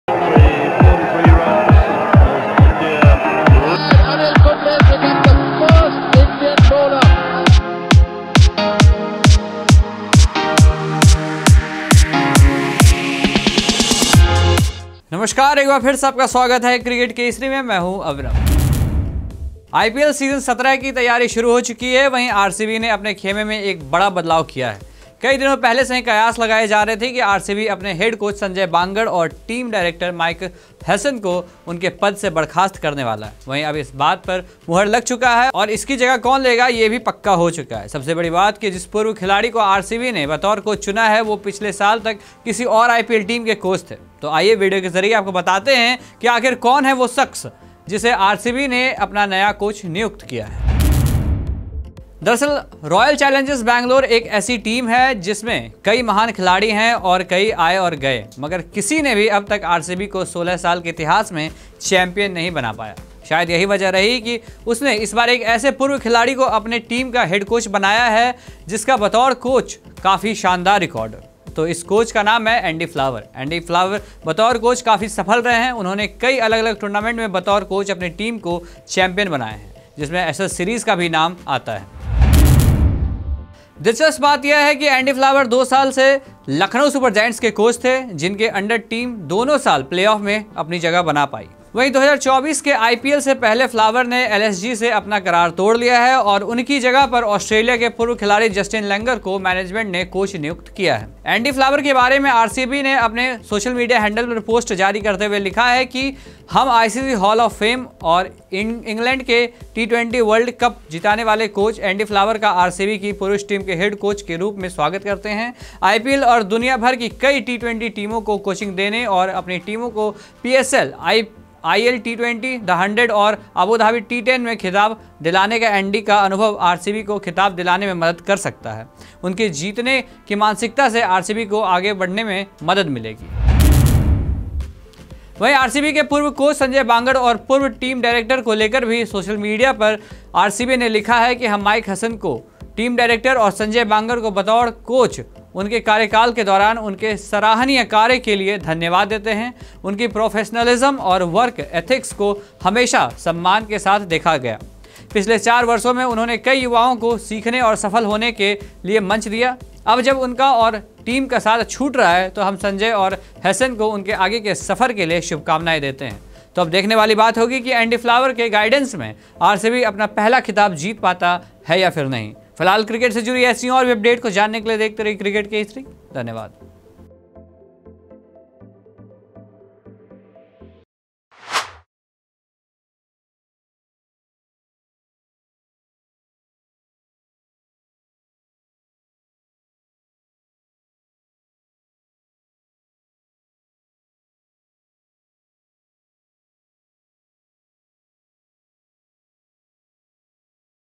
RCB को चैंपियन बनाने के लिए चैंपियन कोच की हुई नियुक्ति, एशेज-PSL में टीम को बना चुके हैं चैंपियन
Preparations for IPL season-17 have started. At the same time, RCB has made a big change in its camp. For several days, speculations were rife that RCB is about to sack its head coach Sanjay Bangar and team director Mike Hesson from their posts.